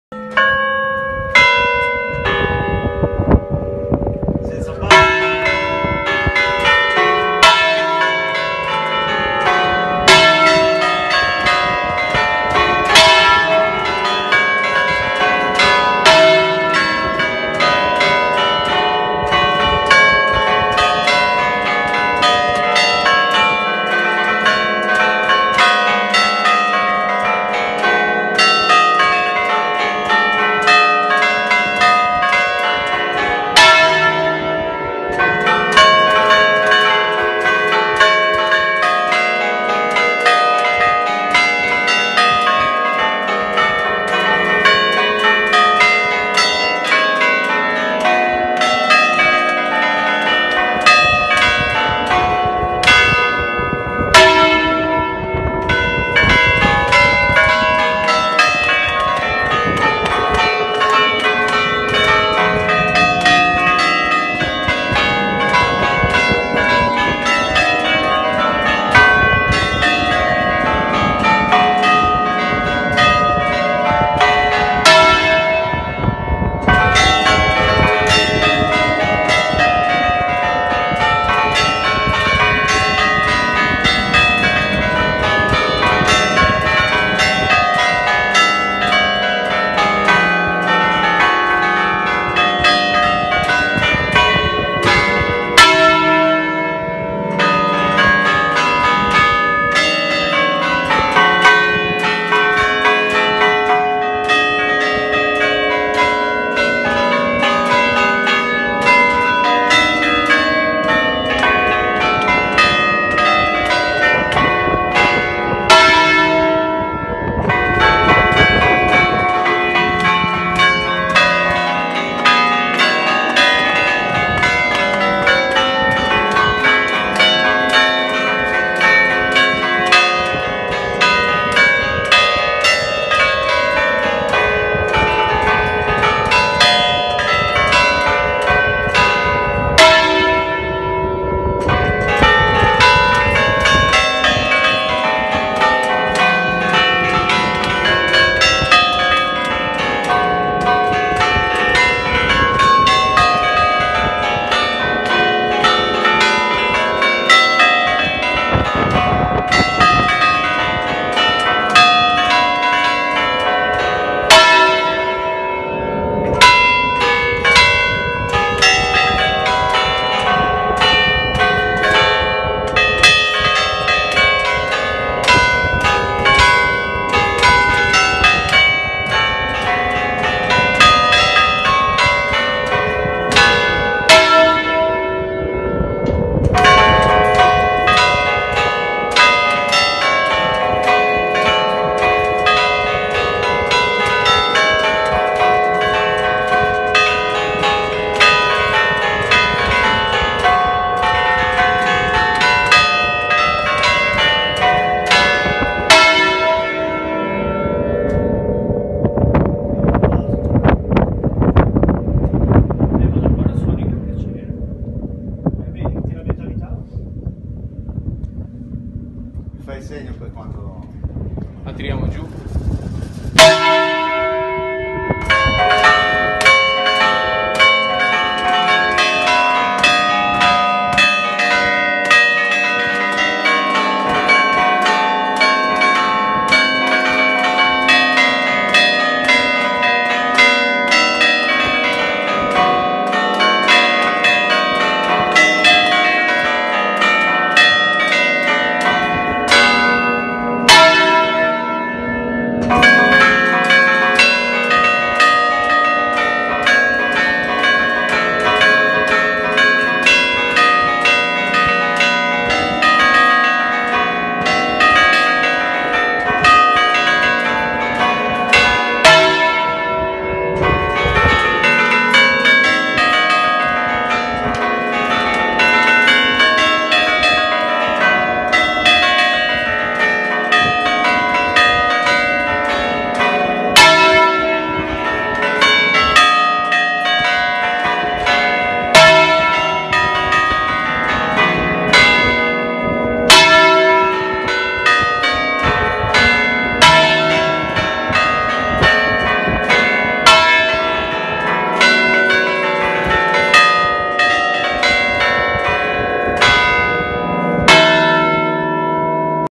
Le campane di Montale di Levanto (Sp)
Pieve di S.Sro di Montale di Levanto (Sp) - 8 campane in MI 3 - Associazione Campanari Liguri in concerto domenica 16 maggio 2021 in occasione della festa del Titolare S.Siro.Suoni solenni con campana "a bicchiere" per la funzione serale